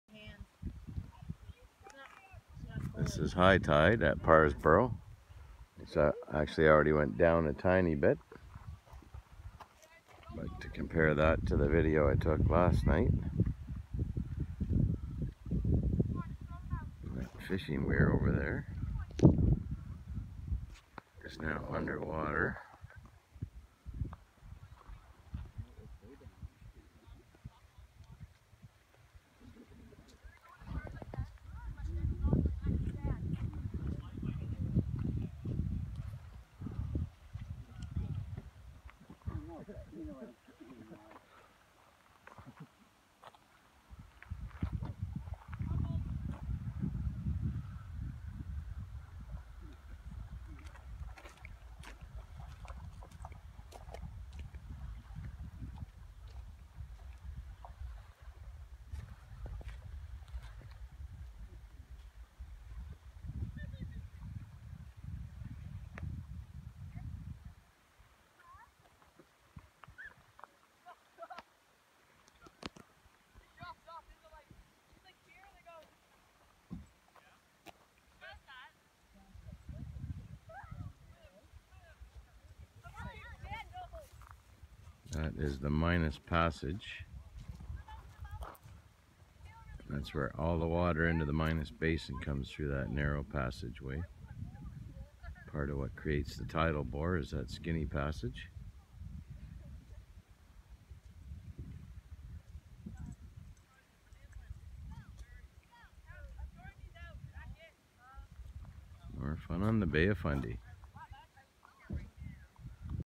High tide at Partridge Island Beach, Parrsboro Nova Scotia.
High tide at the Partridge Island Beach, Parrsboro. The tide today is only 35 ft, but impressive as always. We hiked the trail on the end of the island.